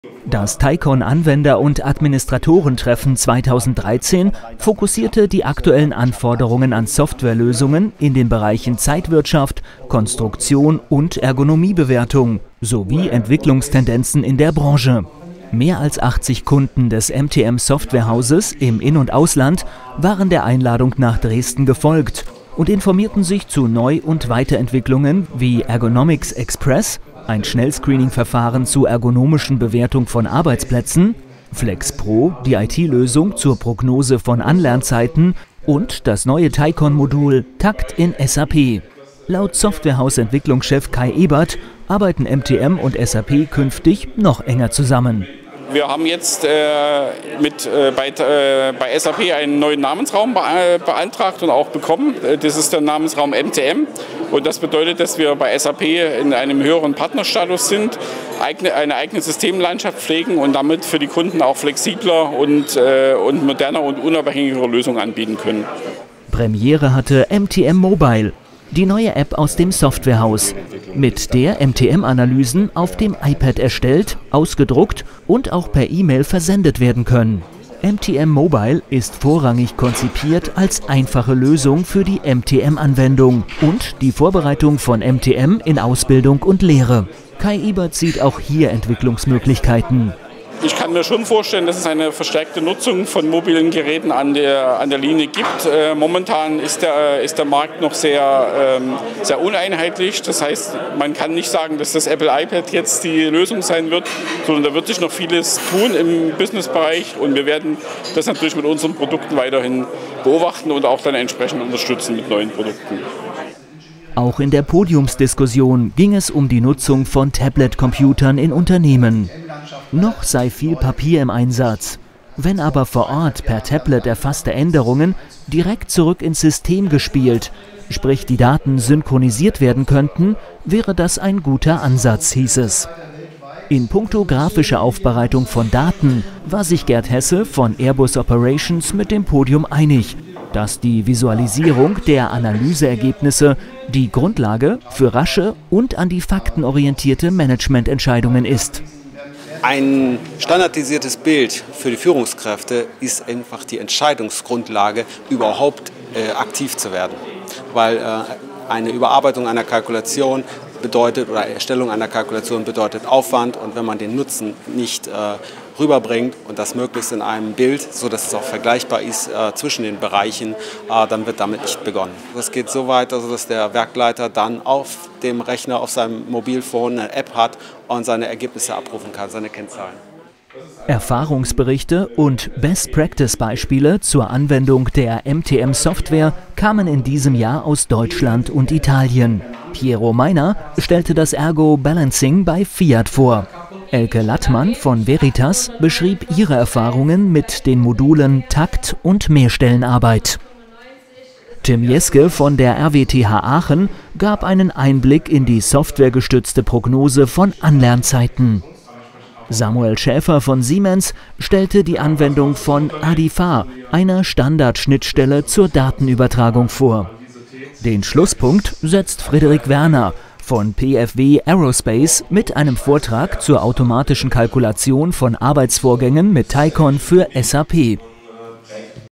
13. TiCon Anwender- und Administratorentreffen - HDready
Mehr als 80 Kunden des MTM-Softwarehauses in In- und Ausland waren der Einladung zum TiCon®-Anwender- und Administratorentreffen 2013 nach Dresden gefolgt. Im Fokus standen innovative IT-Lösungen für Zeitwirtschaft und Ergonomiebewertung.